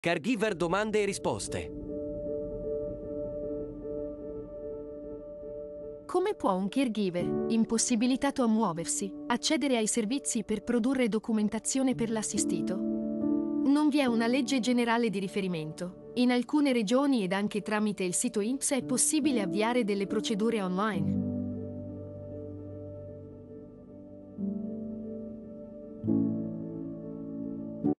Caregiver, domande e risposte- Caregiver impossibilitato a muoversi, come può accedere ai servizi?
cittadinanzattiva.it Caregiver, le tue domande, le nostre risposte.
Dona ora su dona.cittadinanzattiva.it/ 8° domanda